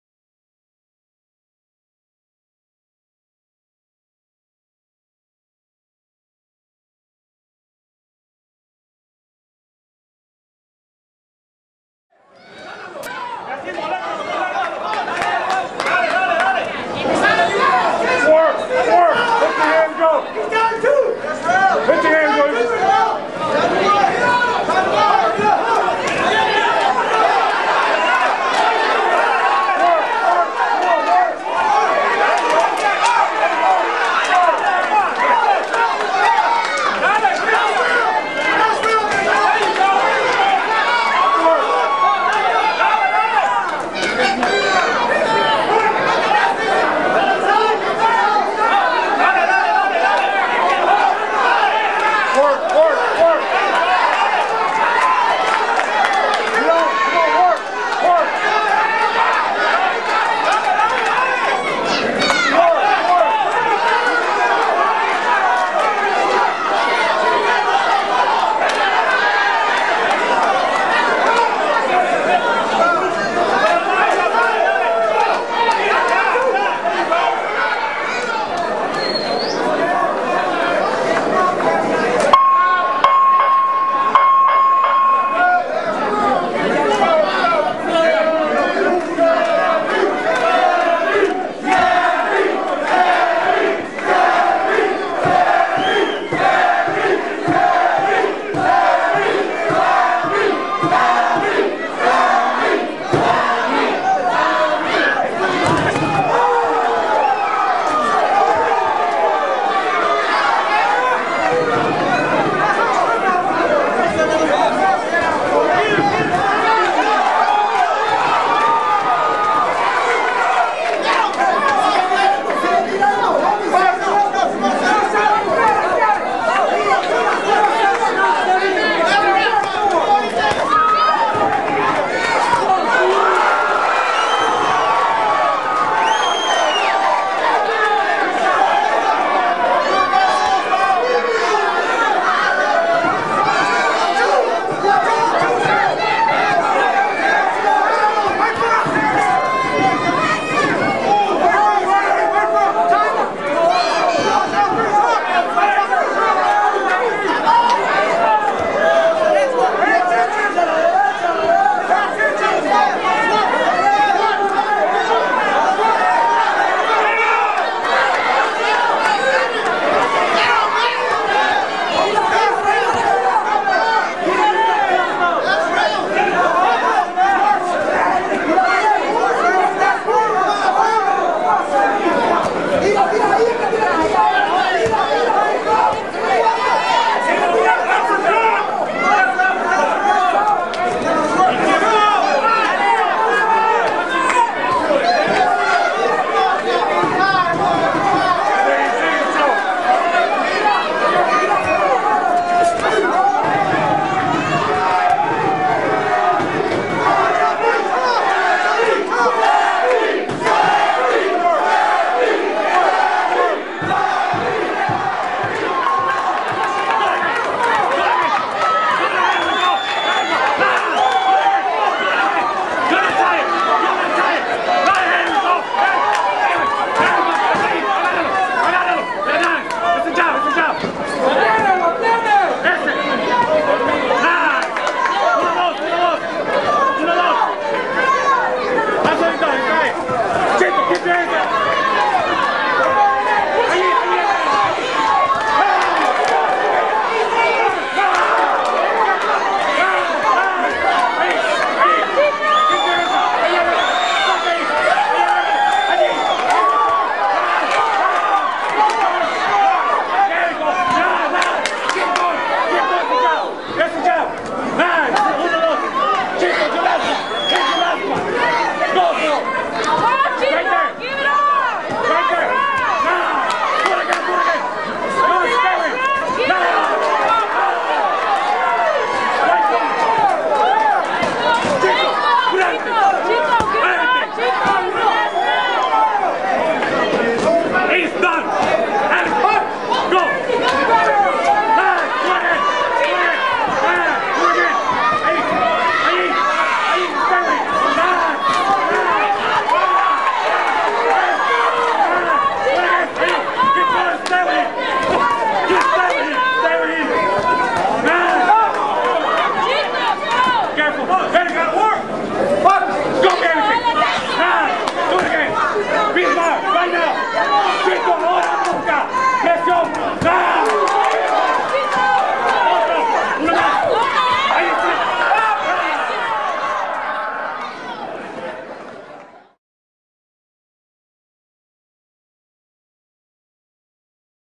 Fight Night at fairgrounds
Dade City Boxing Club hosted seven encounters Saturday, May 11, 2013, at Dan Cannon Auditorium at the Pasco County Fairgrounds.